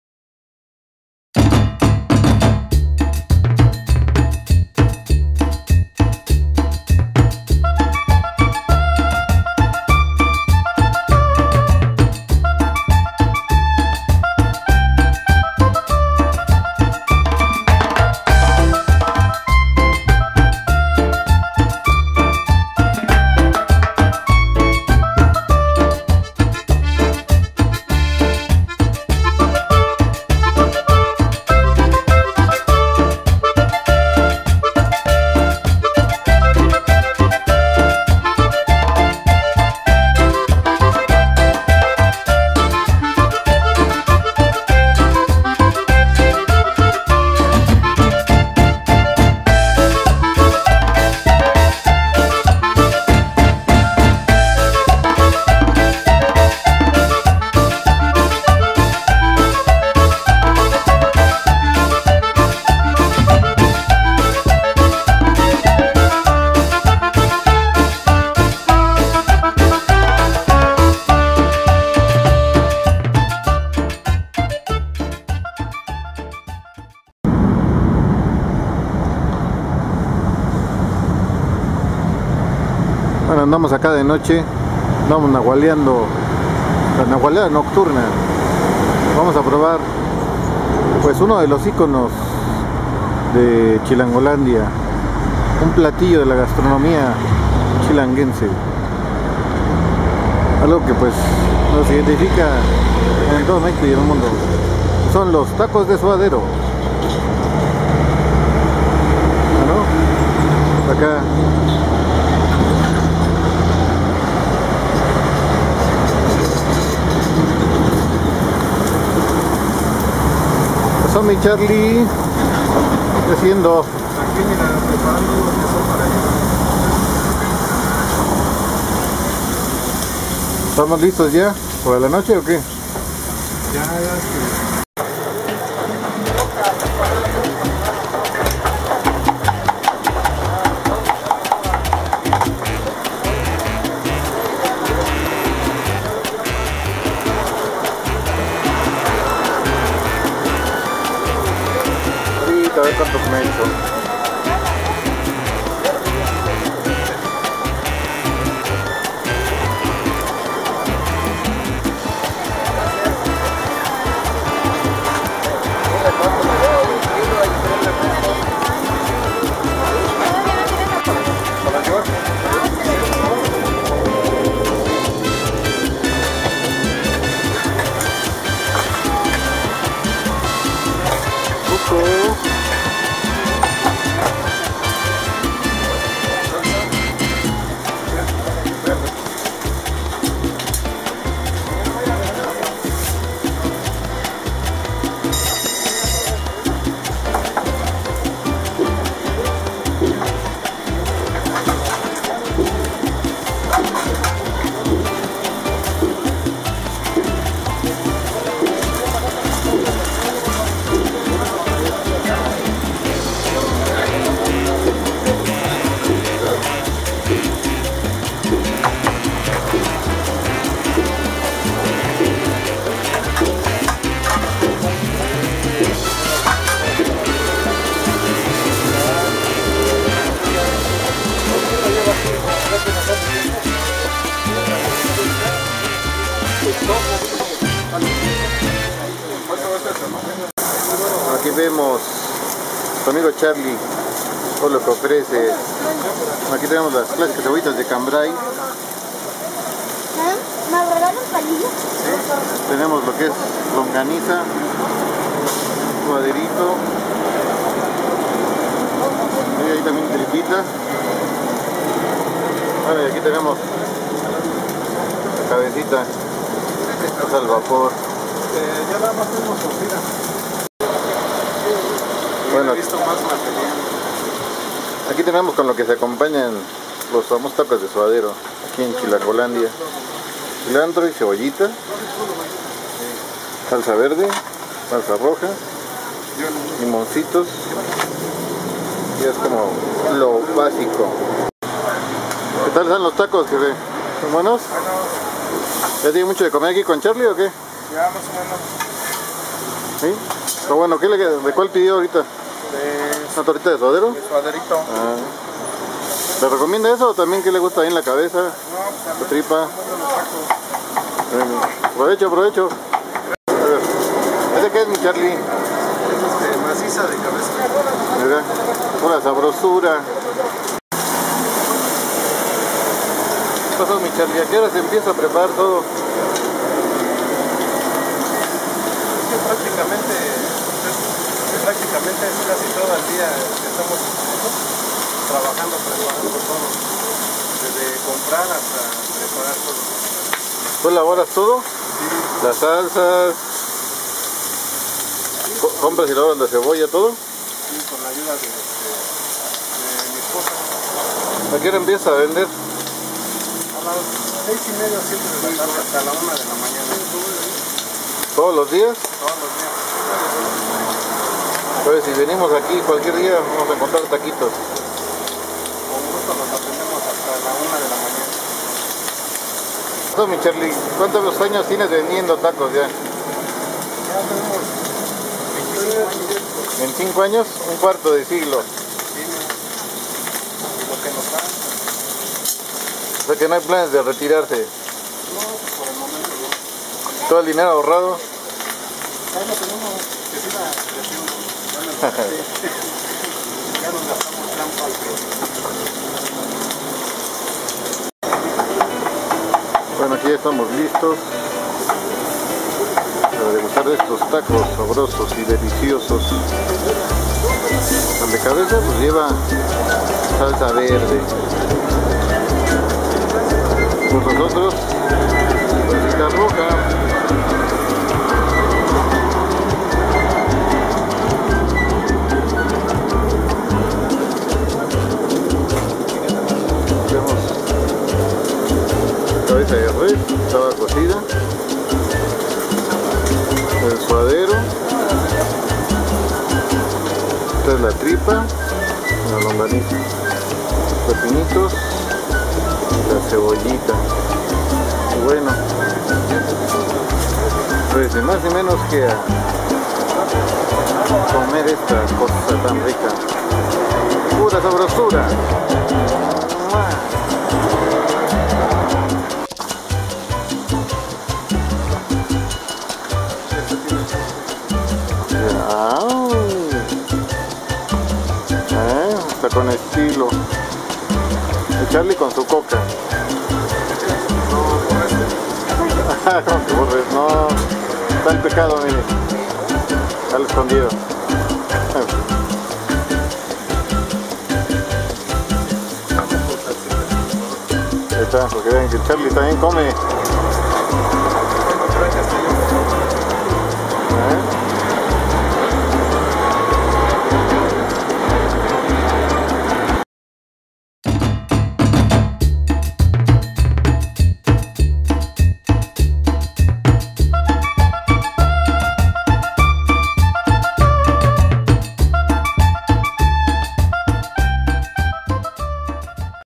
Tacos de suadero(y de cabeza y de tripa y de longaniza) los engordadores del Nahual
Hola Gracias por ver, este puesto ya famoso por los alrededores, se encuentra ubicado entre Vía Tapo, Y avenida Texcoco a una lado de calle Zumpango, de referencia esta muy cerca el mercado Benito Juárez y Avenida Aeropuerto, en la colonia Ciudad Lago Nezahualcoyotl Estado de México, México abajo les dejo el mapa :)